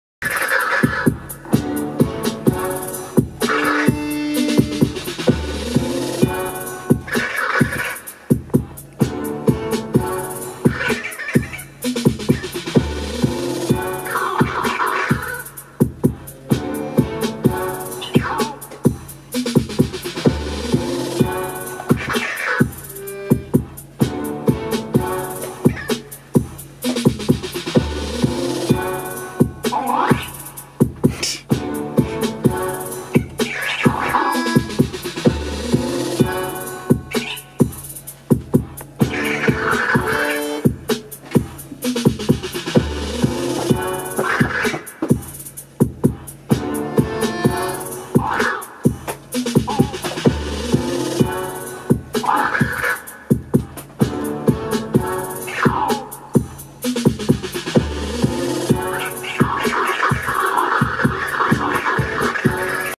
Sami scratch en rythme
Sami utilise le pavé tactile pour scratcher en rythme au Tout Petit Conservatoire. Vous remarquerez qu'il est parfaitement en rythme et calé sur le 2ème temps pour déclencher le scratch, ceci sans aucune indication de ma part. Superbe feeling tellement naturel.